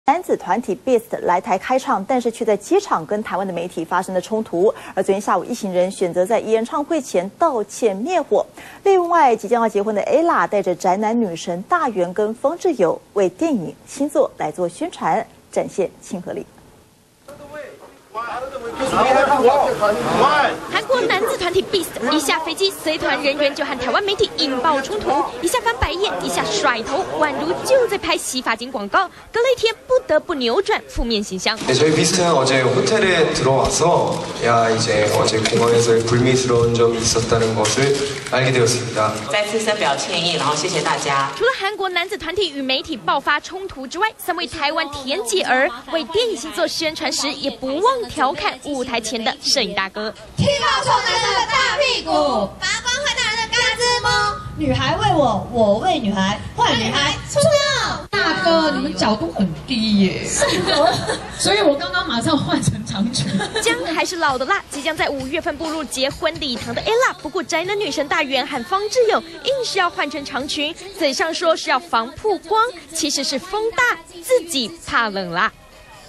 韓團BEAST道歉 Ella性感宣傳
中視新聞》韓國男子團體BEAST來台開唱，卻在機場與媒體發生爆發衝突，隔天立即於演唱會前以鞠躬方式向大眾致歉急滅火；即將為人妻的Ella帶著大元、方志友一起性感宣傳新片。